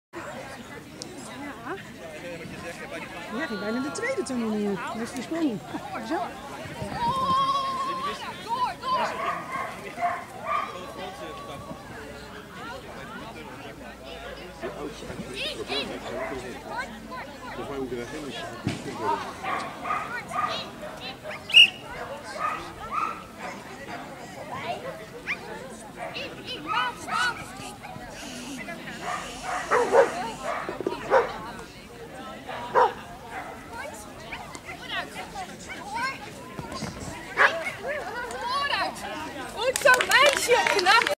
Eager jumping 20-09-2014
Eager at agilityshow in Waddinxveen
Jumping, disqualified due to minor miscommunication, but she was such a good girl!